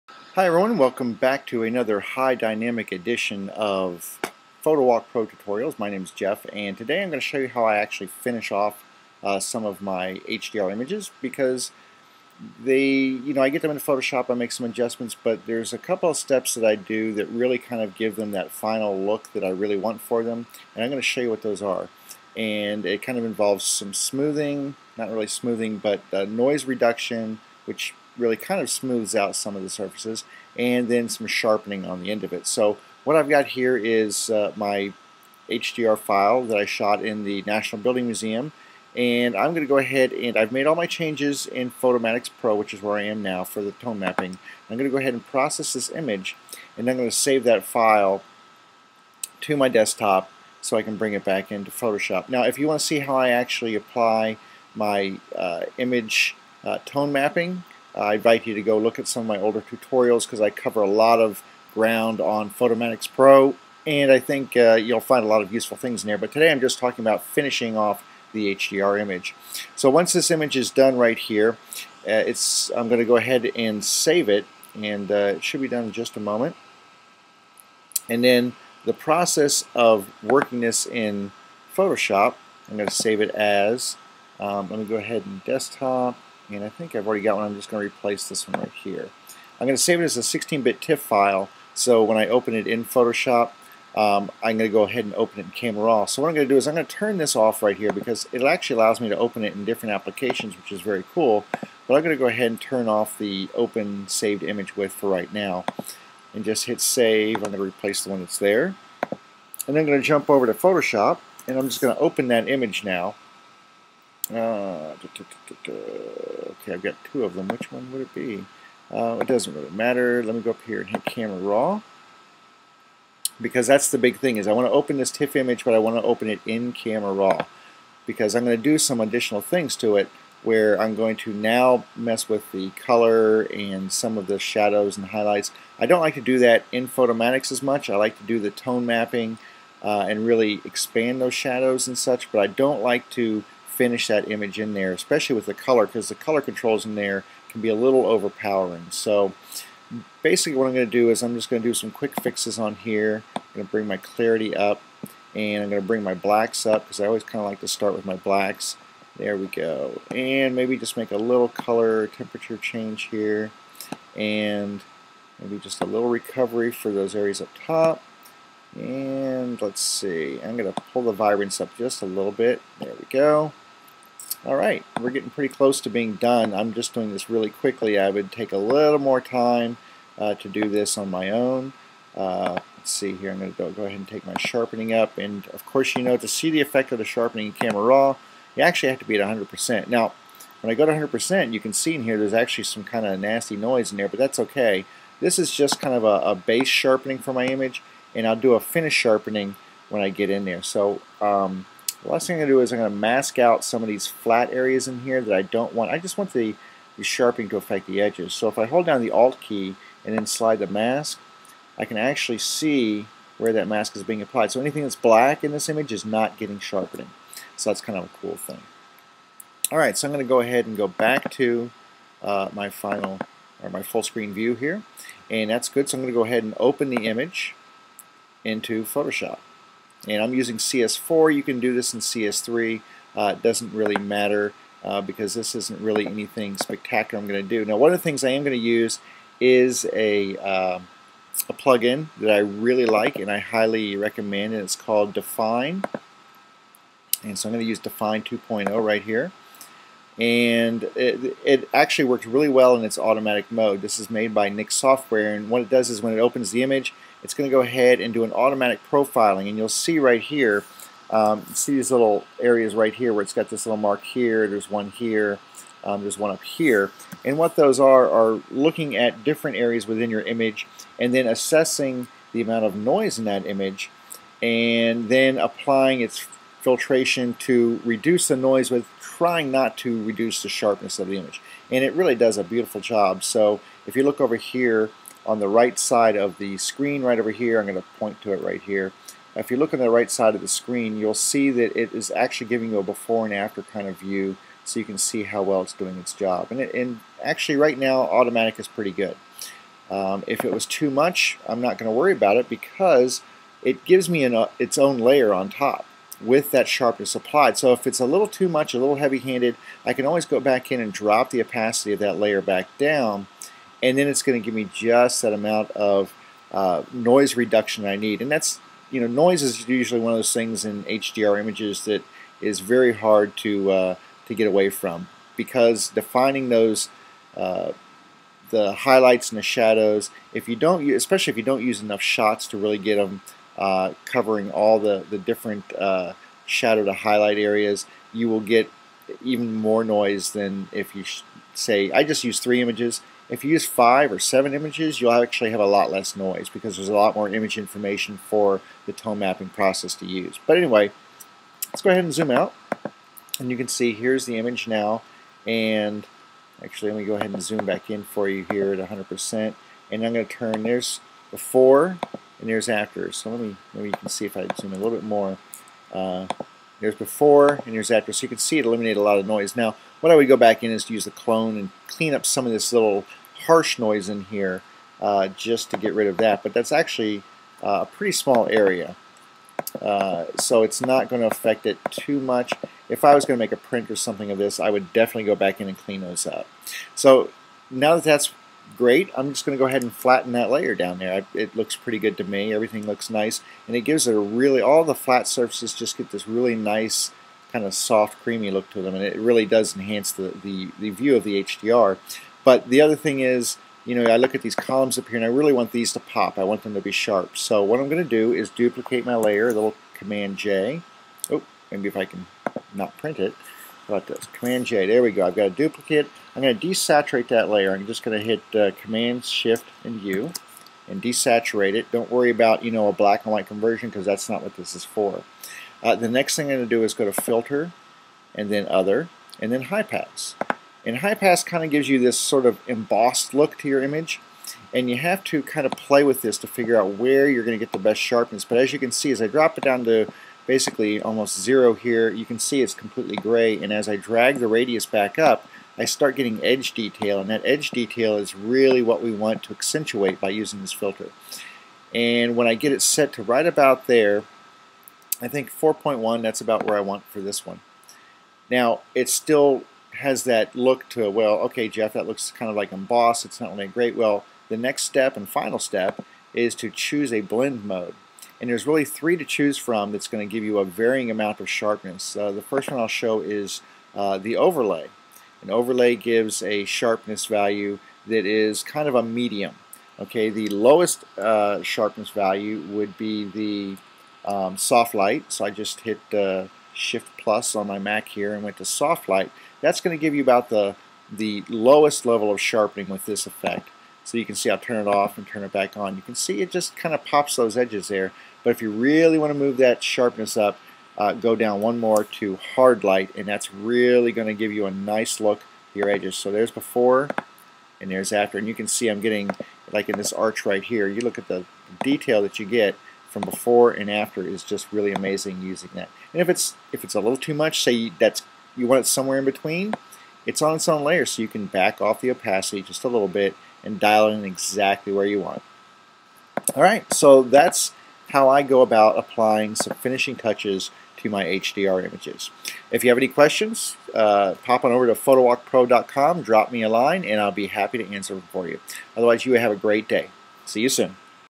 HDR Finishing Touches
In this edition of PhotoWalkPro tutorials I will show you how I apply some finishing touches to my HDR images using the Nik Dfine 2.0 noise reduction plug-in as well as some final sharpening using the High-Pass filter.